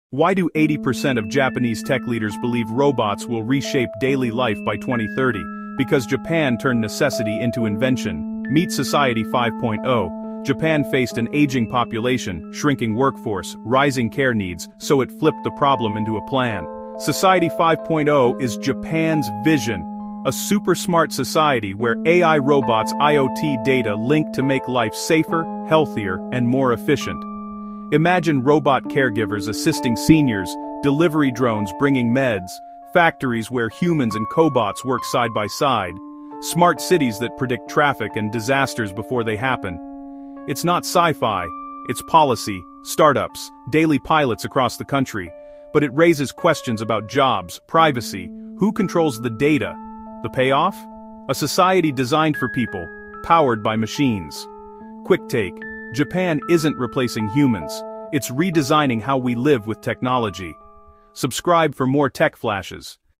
🤖 Society 5.0: Inside Japan’s Robot Nation of the Future (4K Documentary)
Welcome to Japan’s Robot Nation — the birthplace of Society 5.0, where humans and AI live, work, and evolve together. 🇯🇵

In this cinematic short documentary, discover how Japan is transforming into the world’s most advanced digital civilization. From humanoid robots and AI doctors to fully automated cities and smart governance — this is the blueprint for our collective future.

🧠 What You’ll See:

How Japan’s “Society 5.0” merges AI and robotics

Smart cities, self-driving vehicles, and cyber-human integration

The future of jobs, education, and healthcare in AI Japan

📺 Watch till the end for a glimpse into what life could look like by 2050.

VeoVlog3Aiden — Where the Future Becomes Reality.